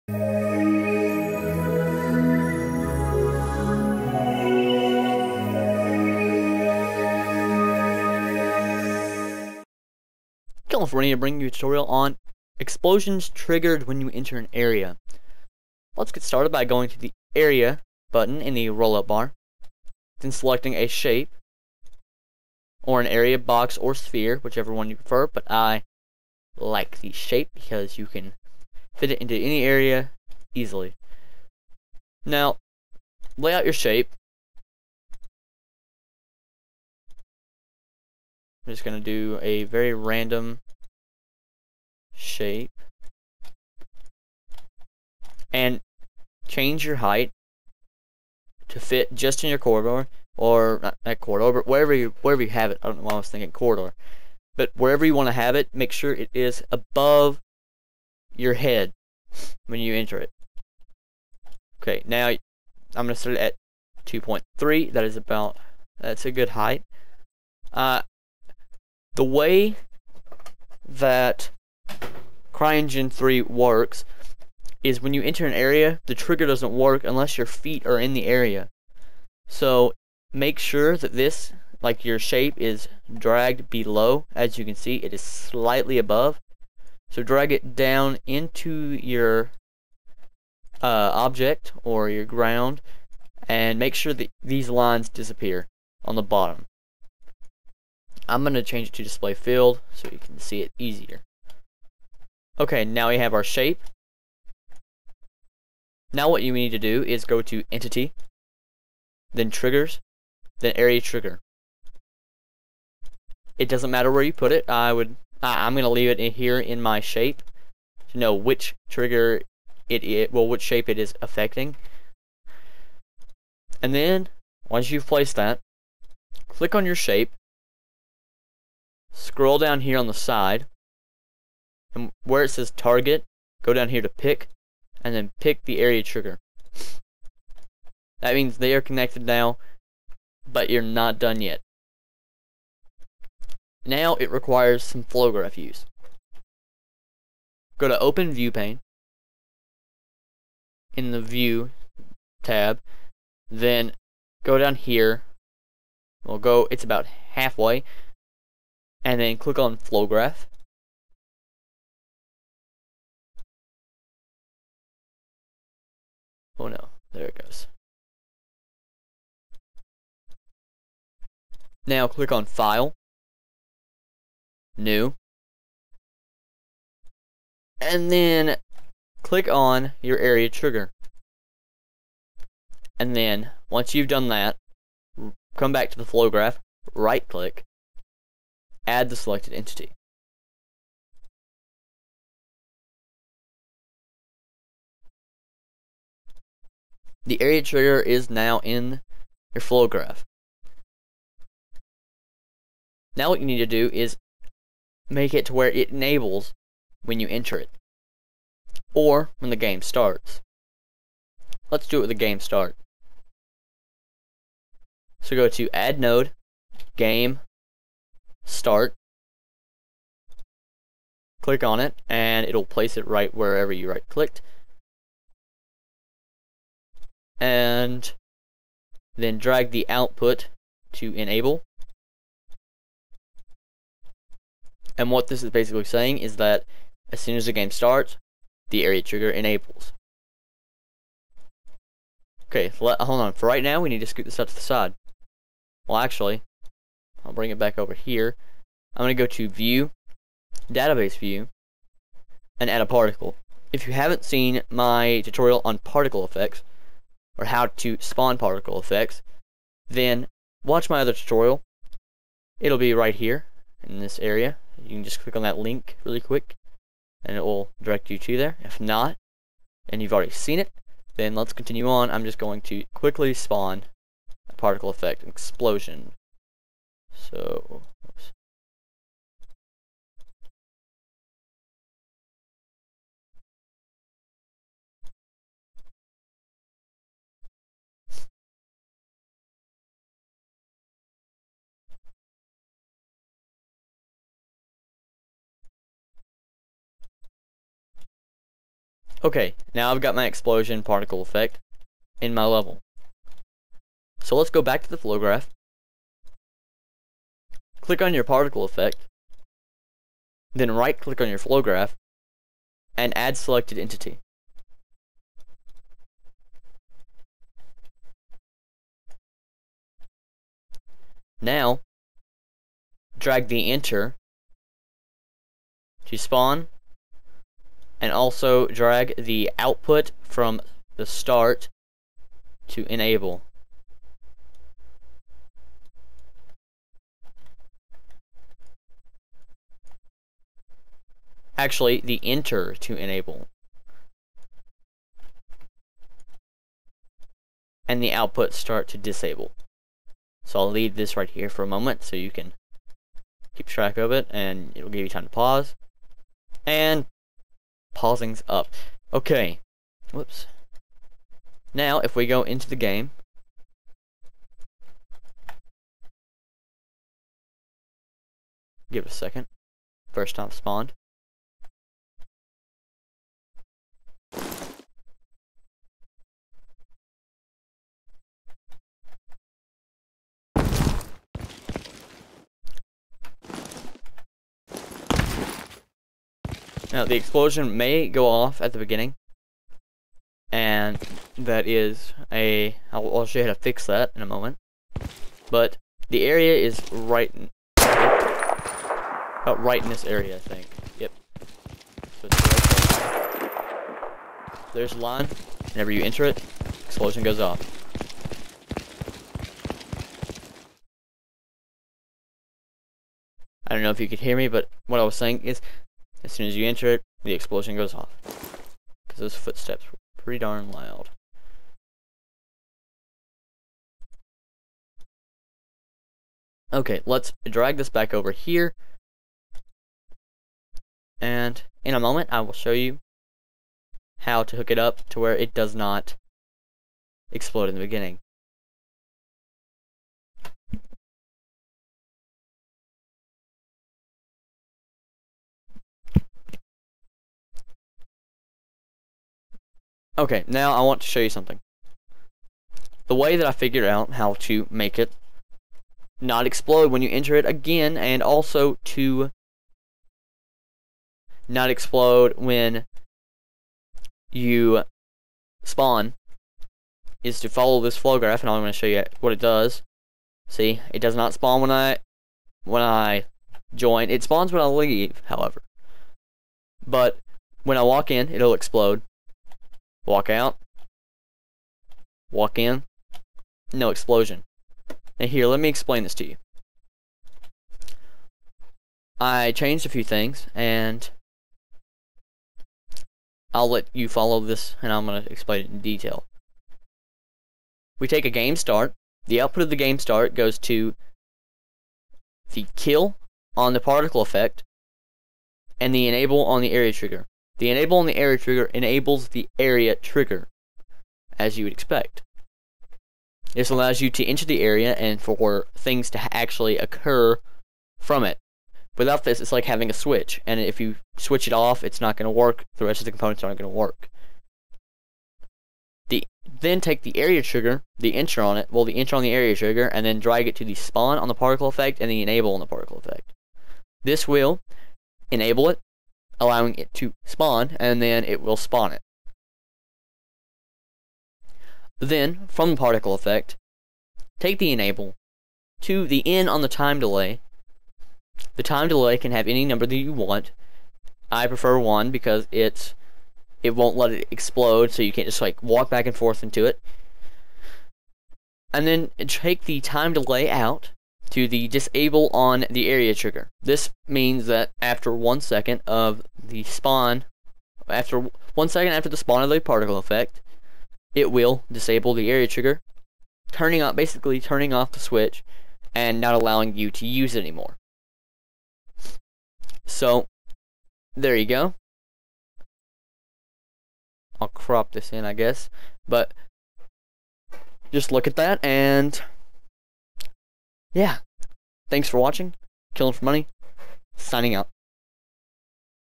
CryEngine 3 - Area Triggered Explosions Tutorial
In this tutorial I show you how to trigger an explosion by entering a specific area!